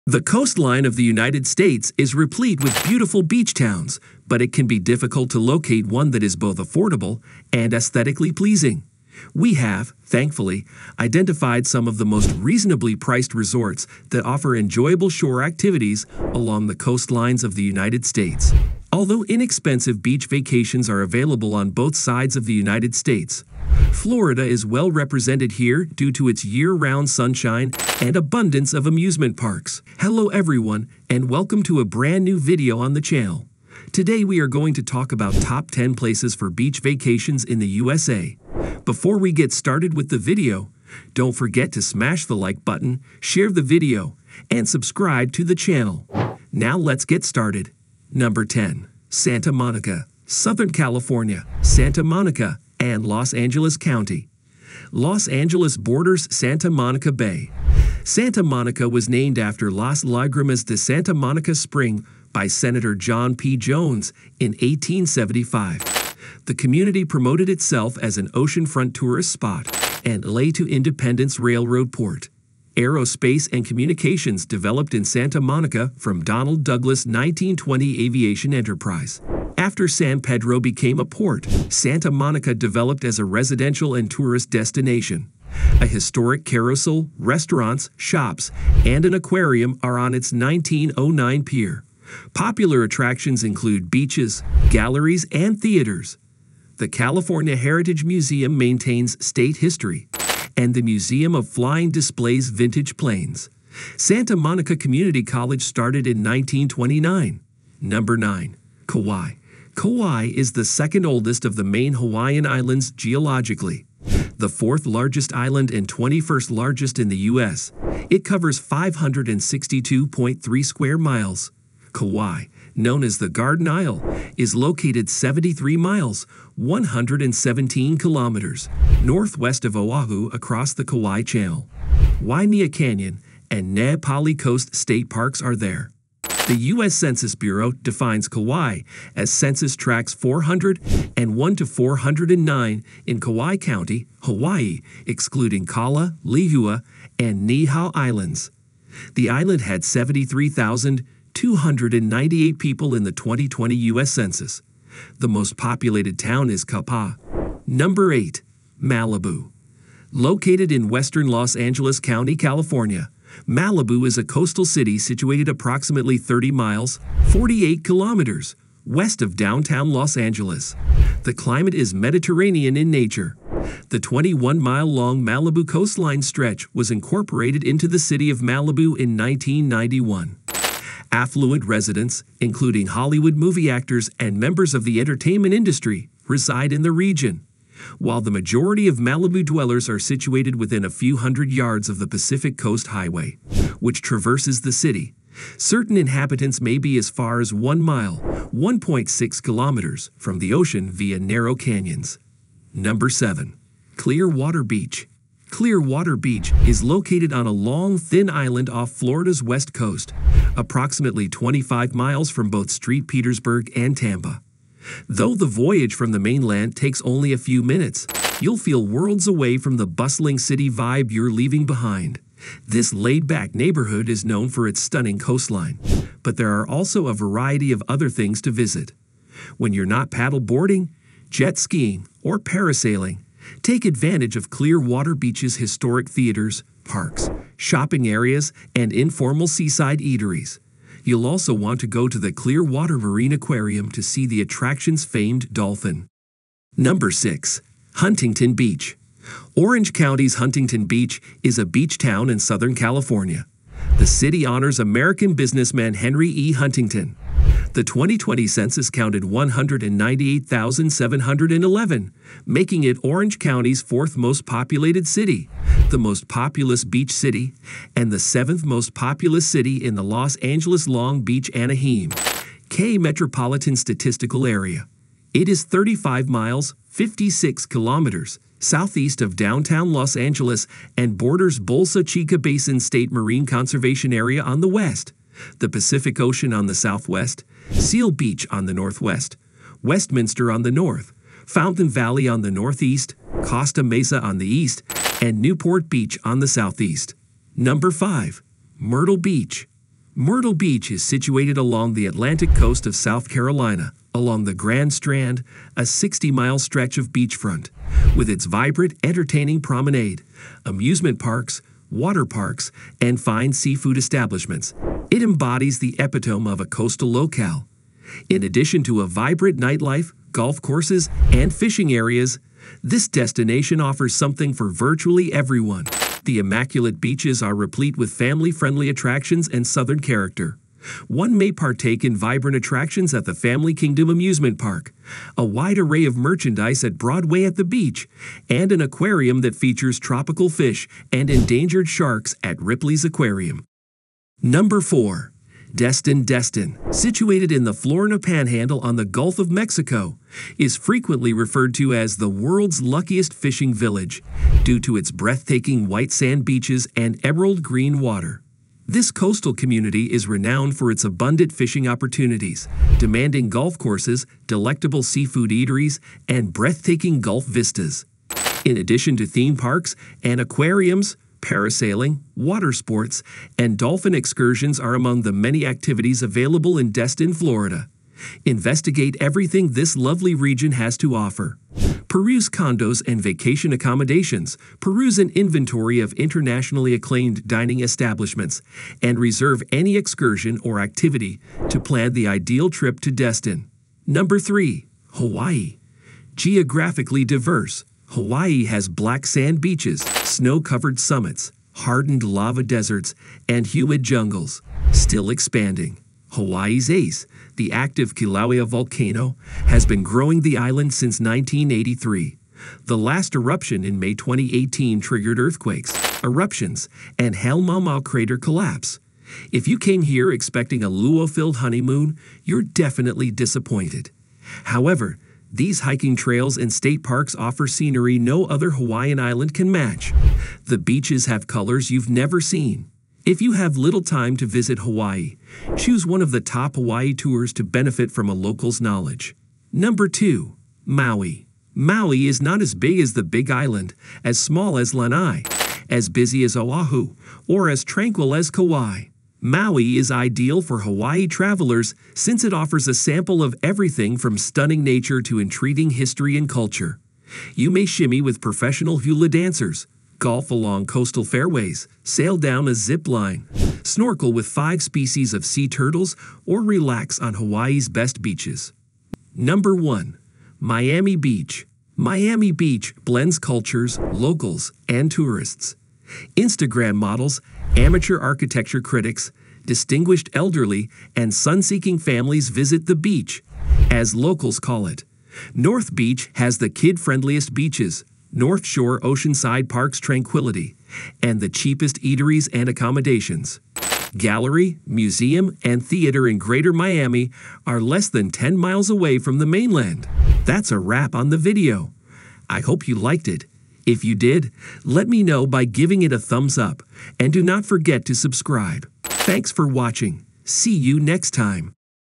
Top 10 places for beach vacations in the USA
The coastline of the USA is replete with beautiful beach towns, but it can be difficult to locate one that is both affordable and aesthetically pleasing. We have, thankfully, identified some of the most reasonably priced resorts that offer enjoyable shore activities along the coastlines of the United States.

Although inexpensive beach vacations are available on both sides of the United States, Florida is well represented here due to its year-round sunshine and abundance of amusement parks. Not to fear if you're seeking a west coast getaway; we've also compiled a list of our favorite California coastal locations.

Hello everyone, and welcome to a brand new video on the channel. Today we are going to talk about  Top 10 places for beach vacations in the USA.